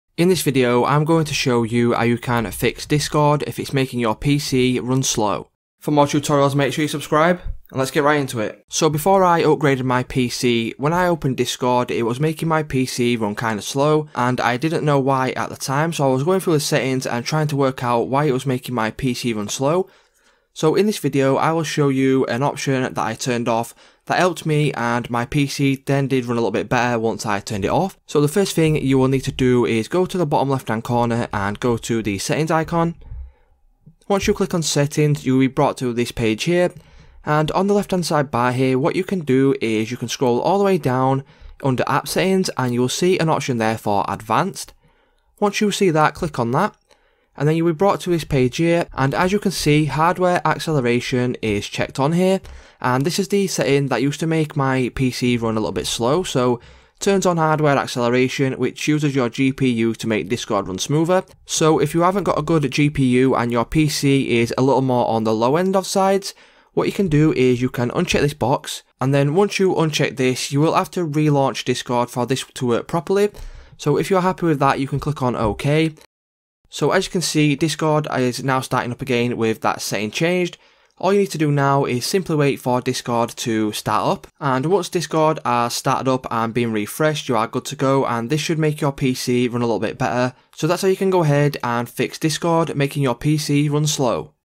Discord Making PC Run Slow FIX! - Stop Discord Making PC Lag
Discord Making PC Run Slow FIX! - Stop Discord Making PC Lag. In this video, I show you how you can stop Discord making your PC run slow and lag. When I turned off this one setting it made my PC run a lot smoother.